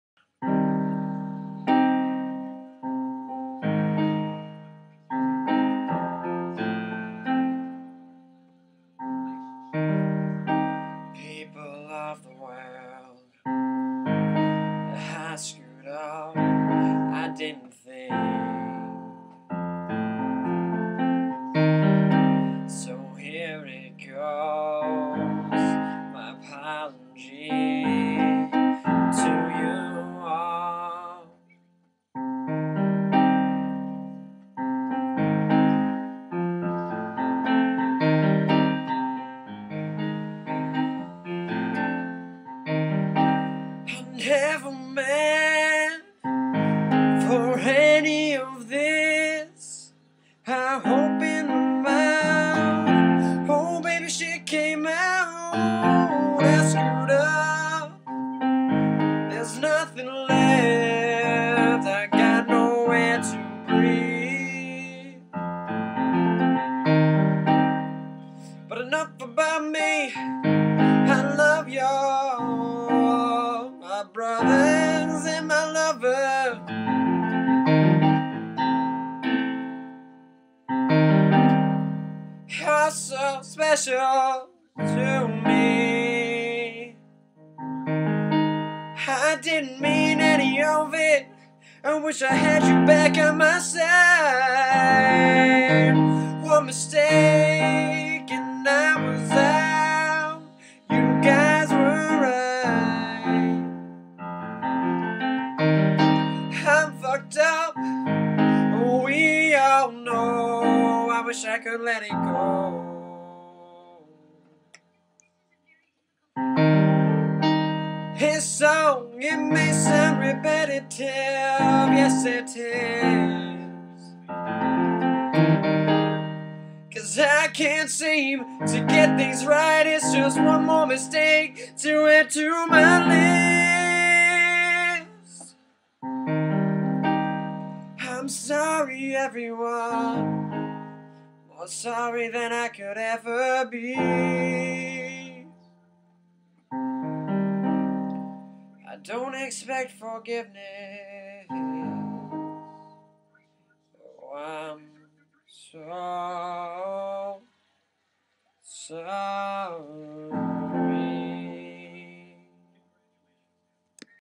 apology song
a song i wrote to my friends that i have been an asshole to. im sorry everyone. no one comment on how bad my singing is. i already know im bad